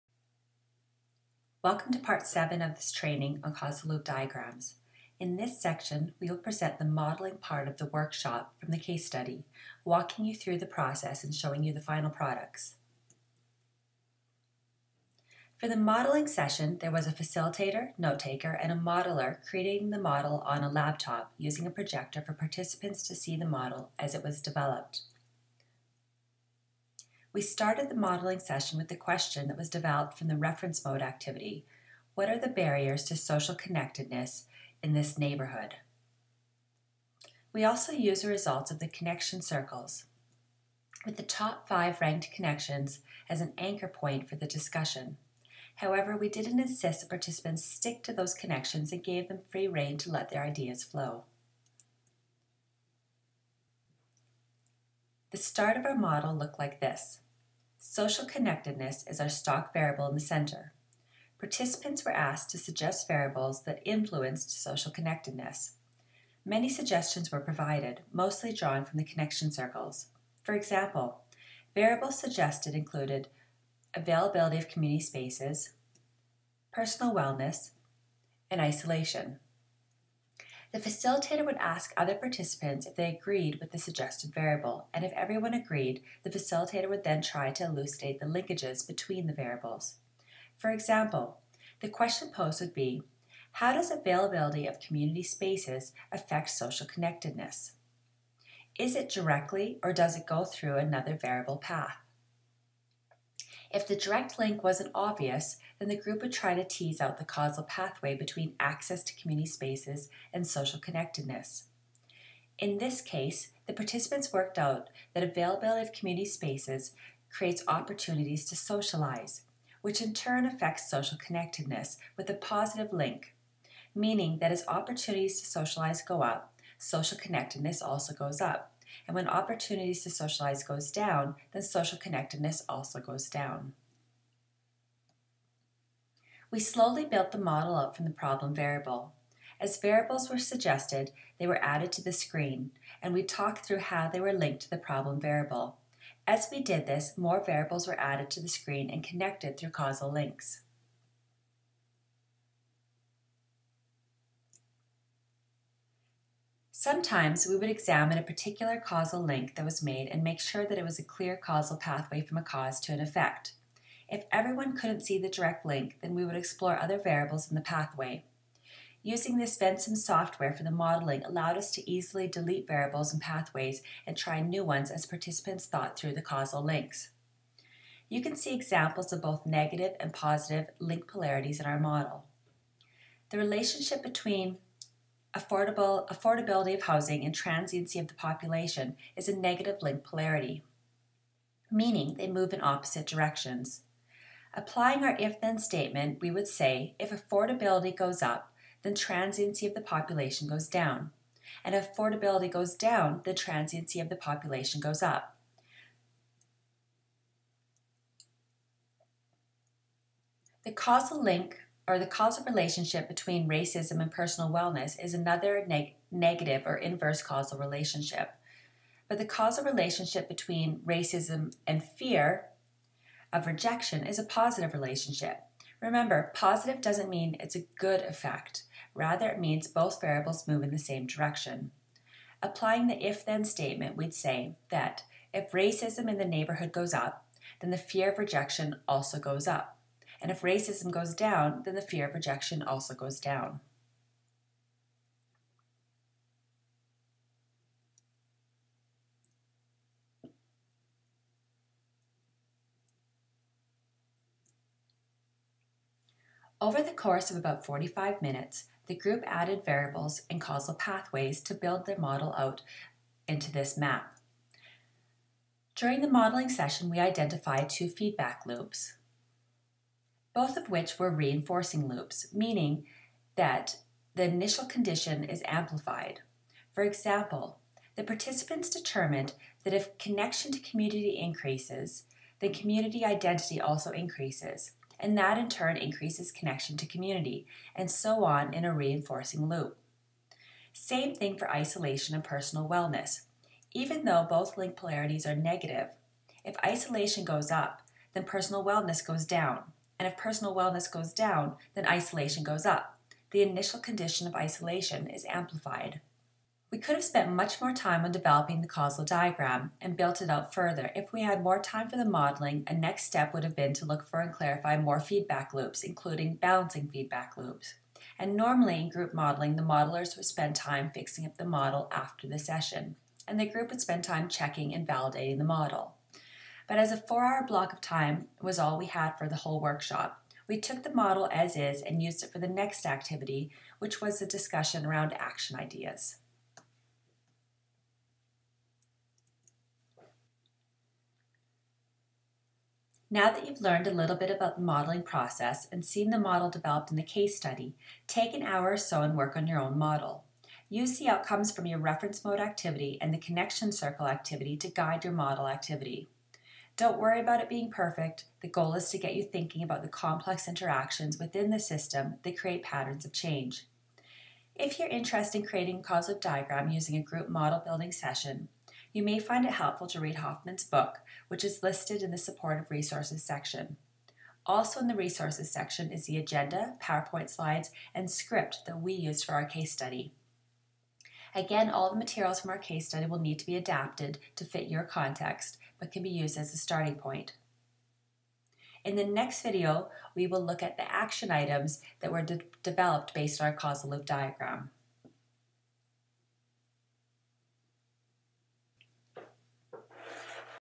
7: Modelling session | Causal Loop Diagrams
This video is part of the "Causal Loop Diagrams" toolbox developed by a Knowledge Translation & Implementation Science Methods Cluster project, "Systems-Thinking Tools for Evidence-Informed Planning"